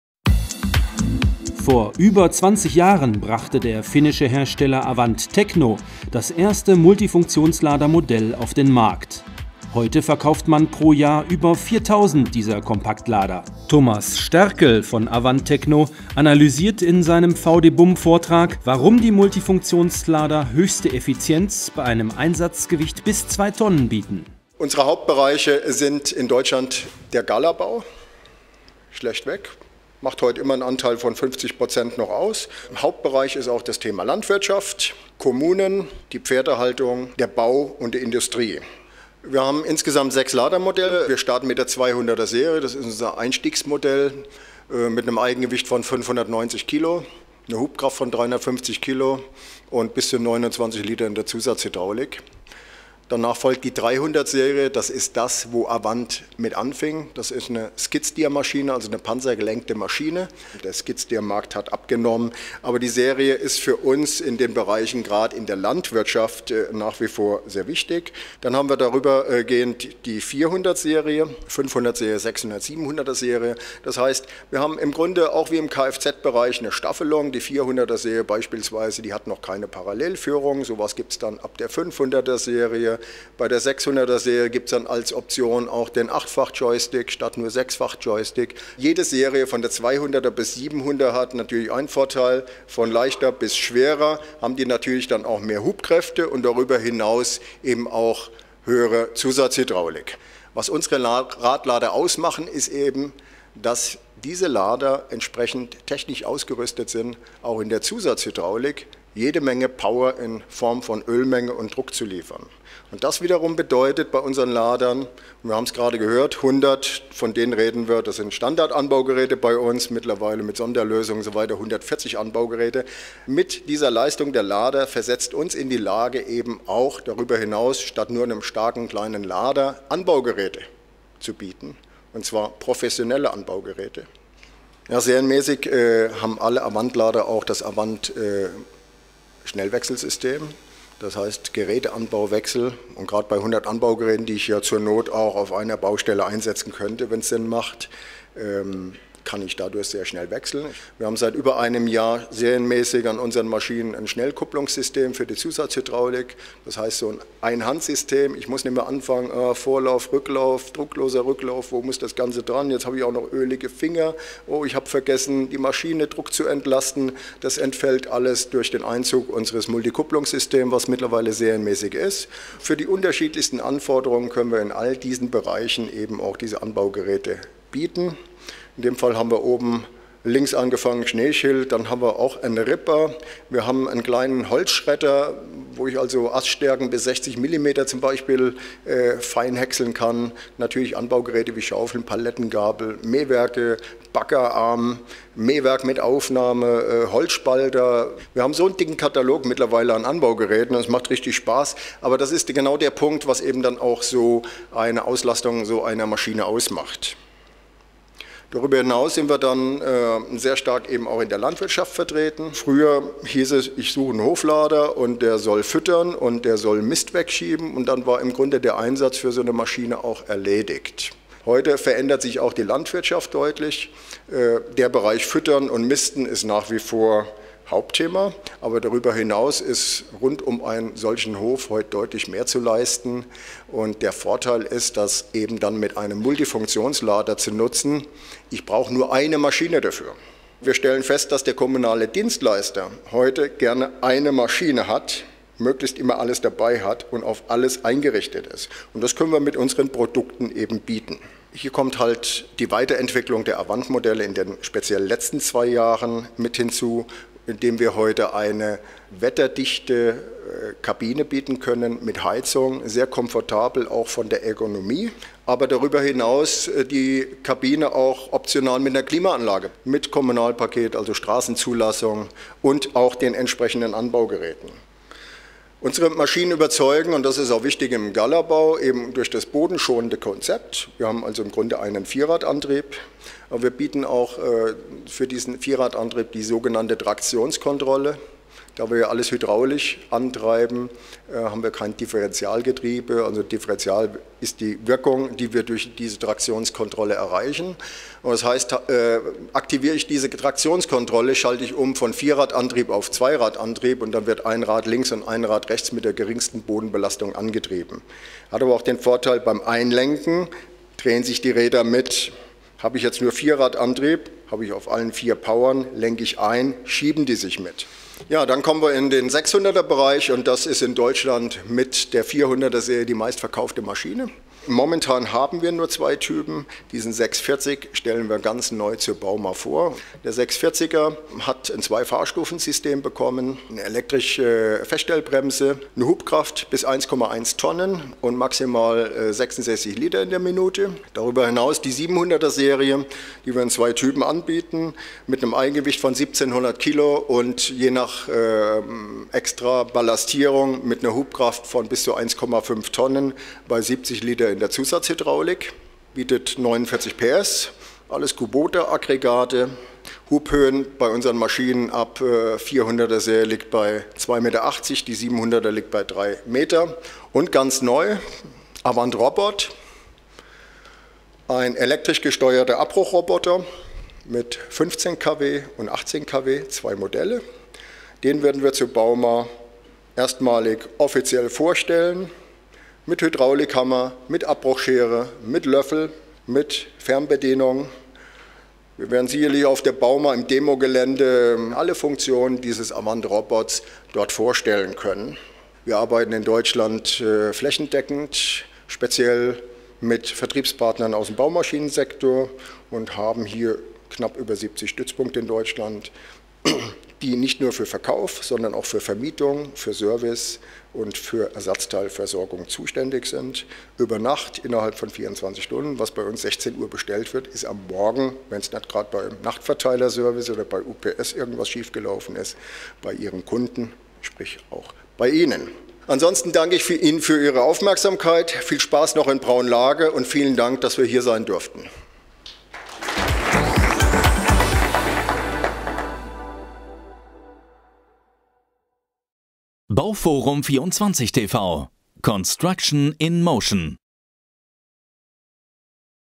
VDBUM Seminar - AVANT Multifunktionslader
Während des diesjährigen 42. VDBUM Großseminars in Braunlage widmete sich ein Sonderseminar dem Thema Garten- und Landschaftsbau. Hier sind kleine, wendige Maschinen gefragt. Was es beim Einsatz der Kompaktmaschinen zu beachten gilt und nach welchen Effizienz-Kriterien man Maschinen für den jeweiligen Einsatz auswählt, davon berichtet Thomas Sterkel von AVANT Tecno in seinem Referat.